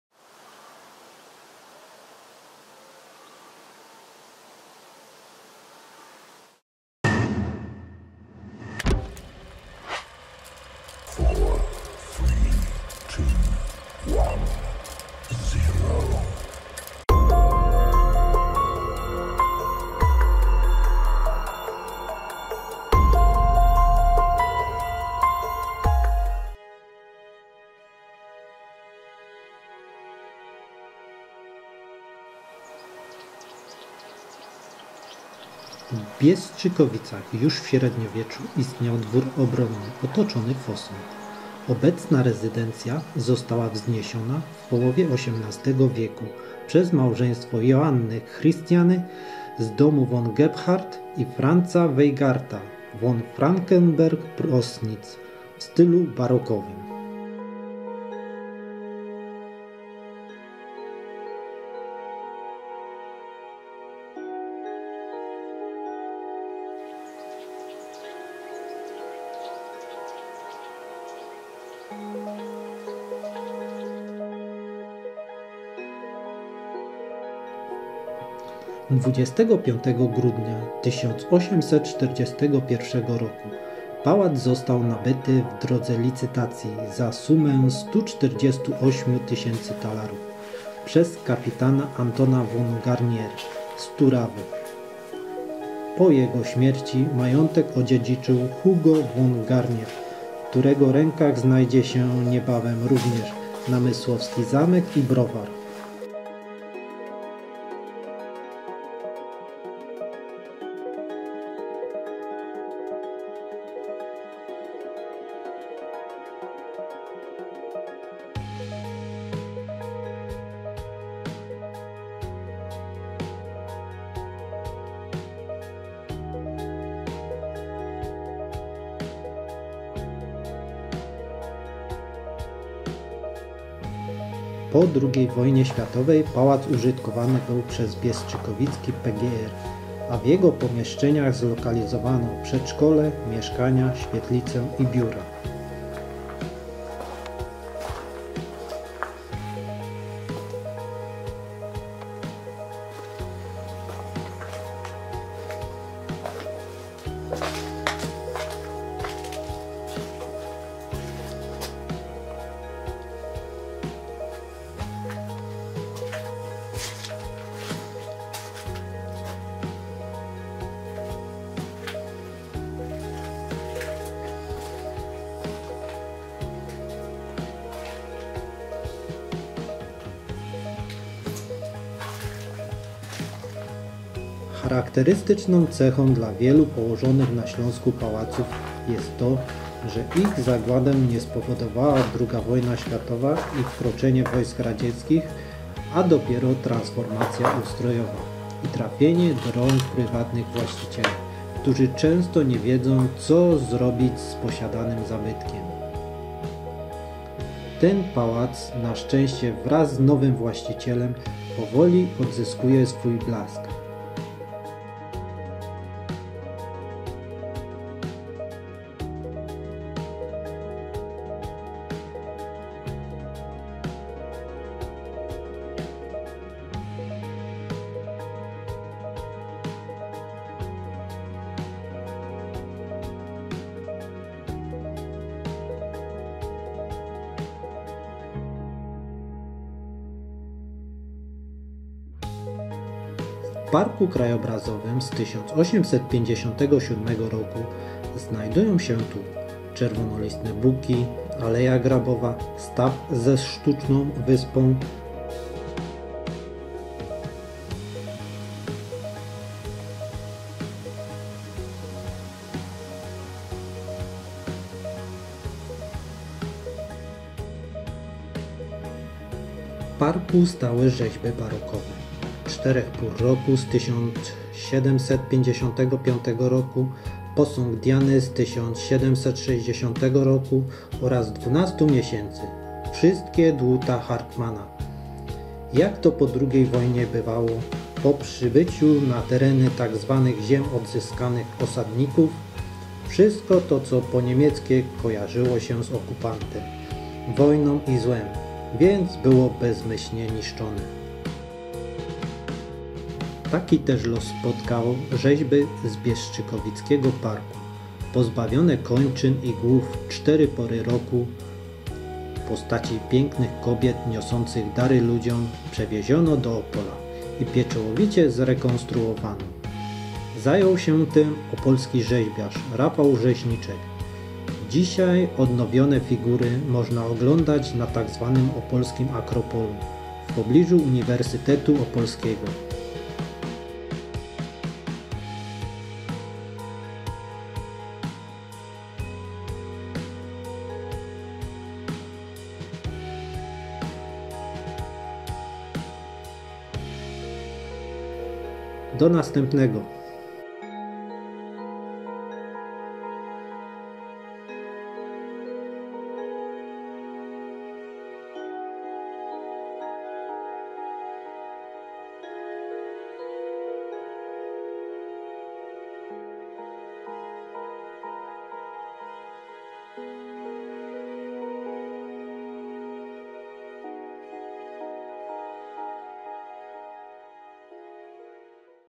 Pałac w remoncie Biestrzykowice
W Biestrzykowicach już w średniowieczu istniał dwór obronny otoczony fosą. Obecna rezydencja została wzniesiona w połowie XVIII w. przez małżeństwo Johanny Chrystiany z domu von Gebhard i Franza Weigarta von Frankenberg–Proschlitz w stylu barokowym.  A 23 grudnia 1841 r. pałac został nabyty w drodze licytacji za sumę 148 tys. talarów przez kapitana Antona von Garnier z Turawy. Po jego śmierci majątek odziedziczył Hugo von Garnier, w którego rękach znajdzie się niebawem również namysłowski zamek i browar. Po II wojnie światowej pałac użytkowany był przez biestrzykowicki PGR, a w jego pomieszczeniach zlokalizowano przedszkole, mieszkania, świetlicę i biura.
Charakterystyczną cechą dla wielu położonych na Śląsku pałaców jest to, że ich zagładę spowodowała nie II wojna światowa i wkroczenie wojsk radzieckich, a dopiero transformacja ustrojowa i trafienie do rąk prywatnych właścicieli, którzy często nie wiedzą co zrobić z posiadanym zabytkiem. Ten pałac na szczęście wraz z nowym właścicielem odzyskuje powoli swój blask.
W Parku krajobrazowym z 1857 roku. Znajdują się tu czerwono listne buki, aleja grabowa i staw ze sztuczną wyspą. W parku stały rzeźby barokowe Czterech Pór Roku z 1755 roku i posąg Diany z 1760 oraz Dwunastu Miesięcy wszystkie dłuta Hartmanna . Jak to po II wojnie bywało, po przybyciu na tereny tak zwanych. Ziem Odzyskanych osadników, wszystko to co "niemieckie" kojarzyło się z okupantem, wojną i złem, więc było bezmyślnie niszczone. Taki też los spotkał rzeźby z biestrzykowickiego parku. Pozbawione kończyn i głów Cztery pory roku - postaci pięknych kobiet, niosących dary ludziom przywieziono do Opola i pieczołowicie zrekonstruowano. Zajął się tym opolski rzeźbiarz - Rafał Rzeźniczek. Dzisiaj odnowione figury można oglądać na tak zwanym Opolskim Akropolu, w pobliżu Uniwersytetu Opolskiego.